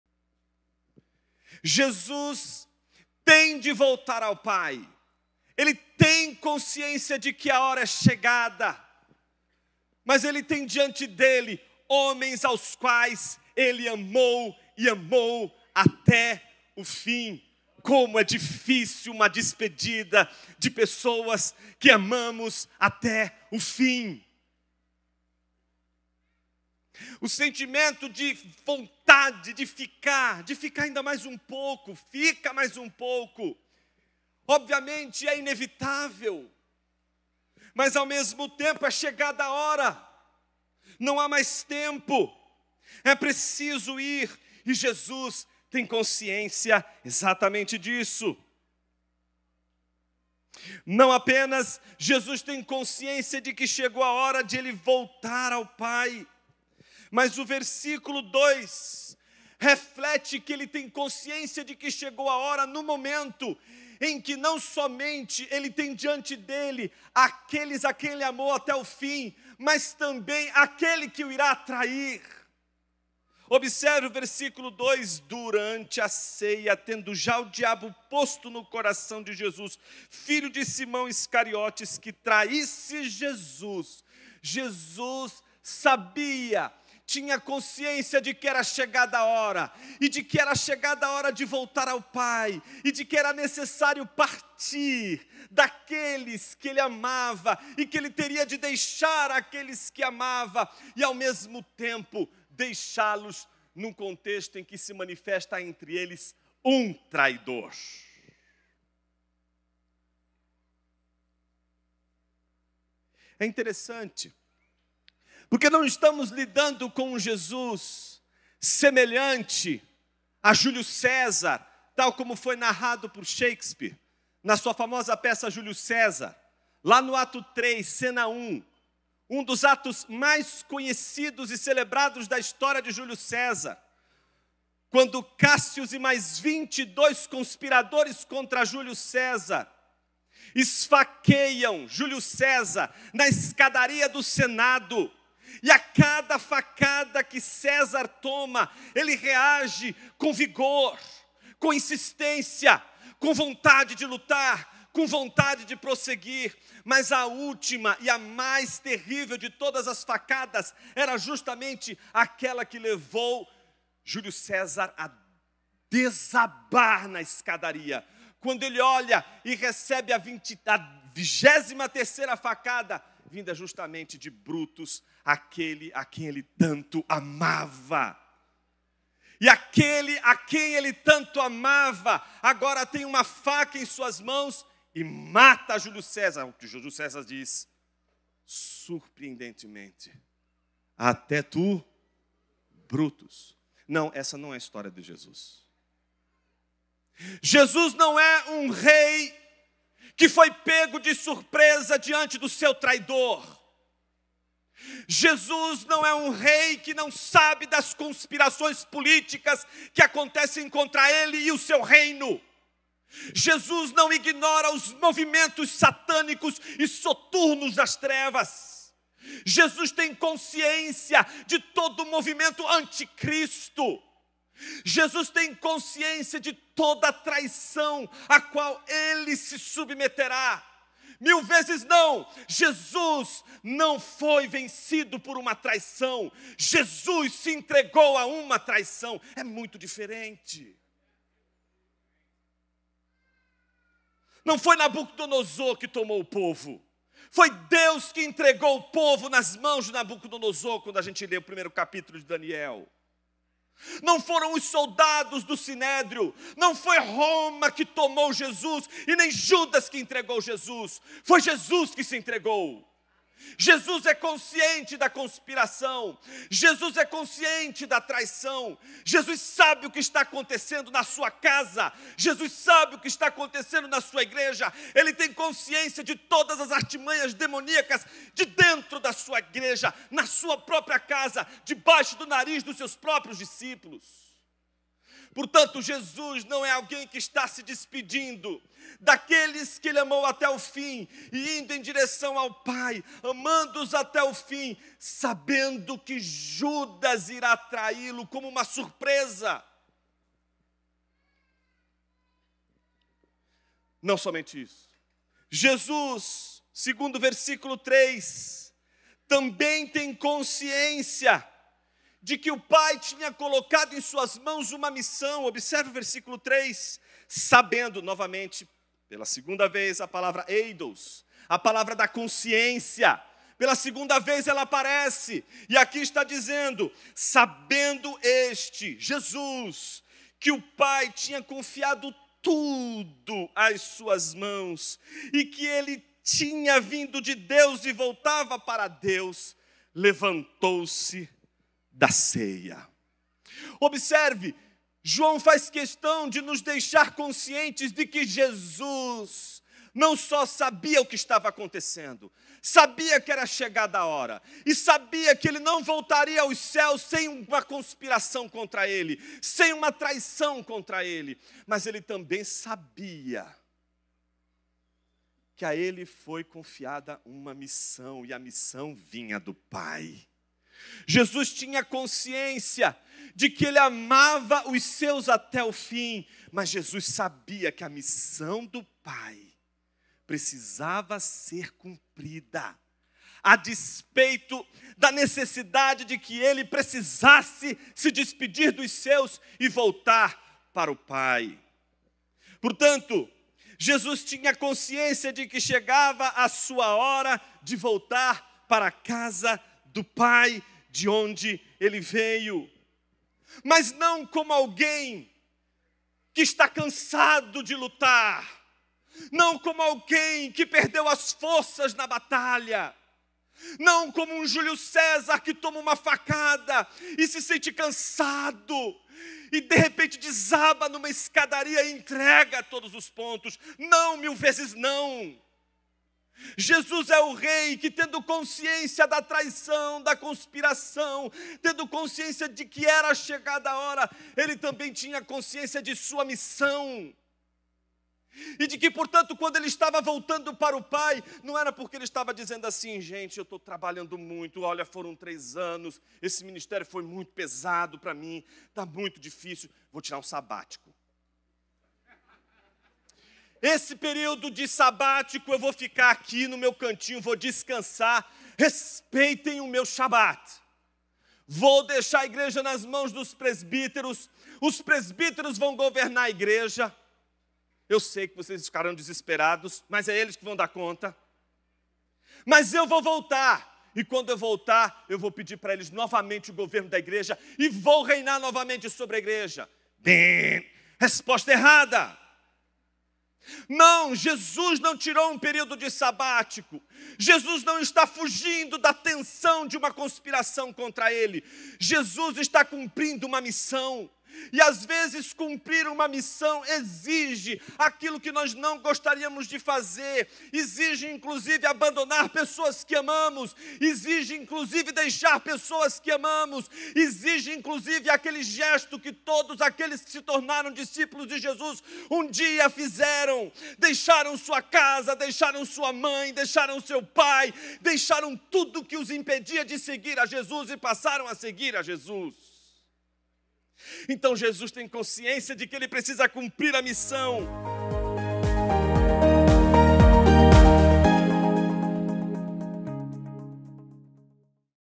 PLENA CONSCIÊNCIA DA MISSÃO | Jonas Madureira
A missão delegada a Cristo de vir redimir Seu povo foi acompanhada de muito sofrimento. Cristo enfrentou as dores comuns dos homens, enfrentou a dor da despedida de quem Ele amou até o fim e, principalmente, a maior dor, a da cruz!

O nosso Senhor, como a palavra nos apresenta, sabe o que é sofrer e padecer; mas o que nos traz consolo é saber que nada disso foi capaz de fazer o Cristo desistir, havia plena consciência da necessidade e importância da missão, Ele tinha consciência de tudo o que haveria de enfrentar, como também, que o Seu sofrimento haveria de resultar em glória.

Não se engane, toda a história que conhecemos é conhecida pelo Criador, há consciência, soberania e onipresença em todas as esferas do tempo regidas por Ele. Confie neste ensino!

Confira um dos momentos da pregação ministrada pelo pastor Jonas Madureira durante a Consciência Cristã 2023!